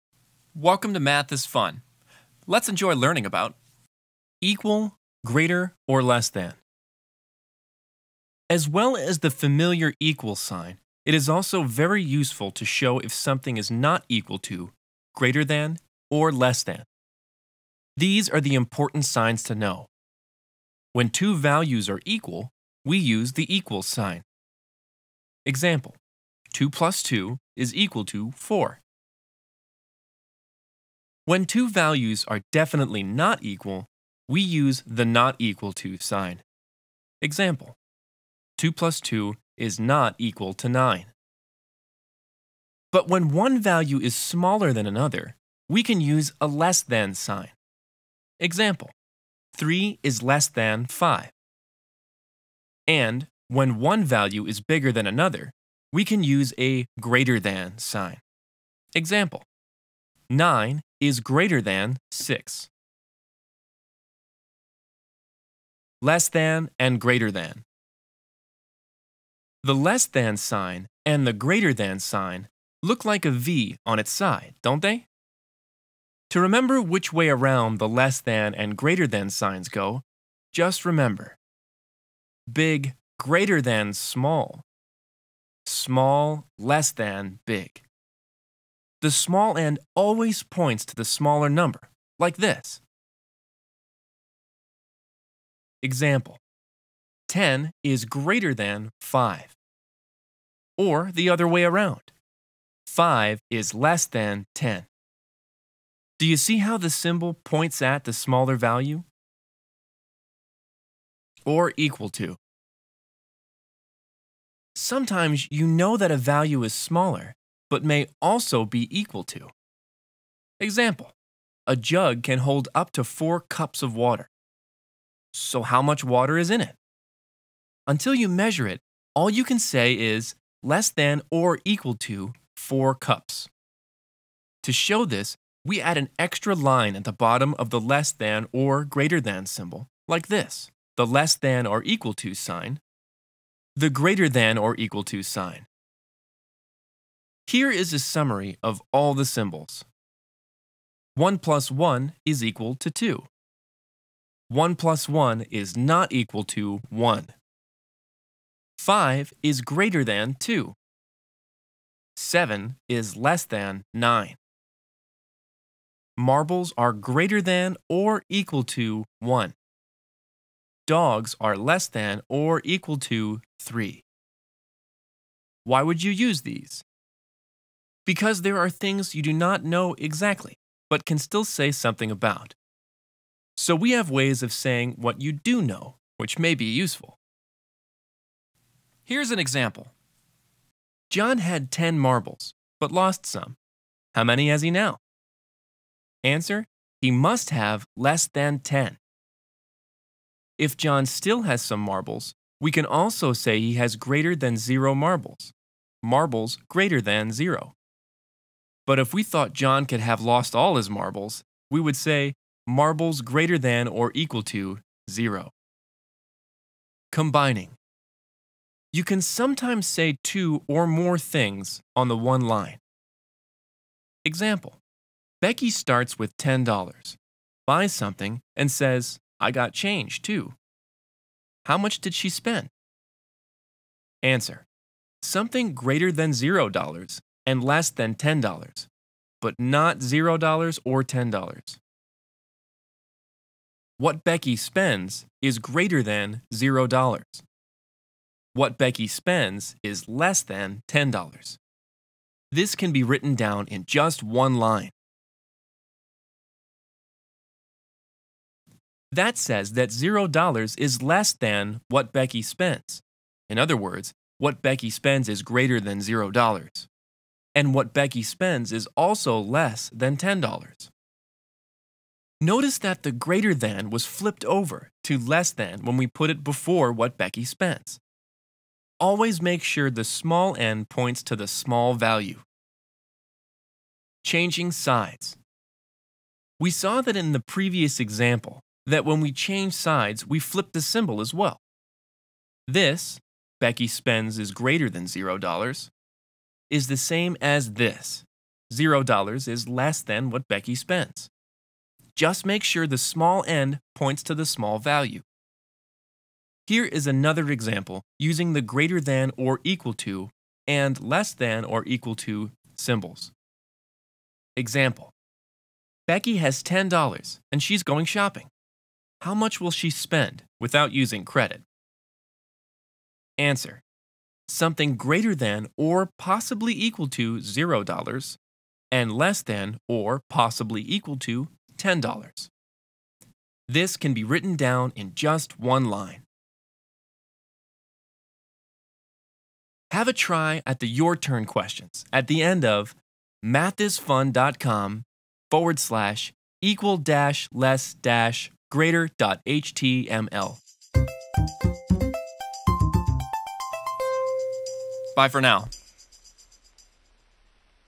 Equal, Greater or Less Than | Math is Fun
As well as the familiar equals sign (=) it is also very useful to show if something is not equal to, greater than, or less than.

Clear explanations with examples make it easy to understand.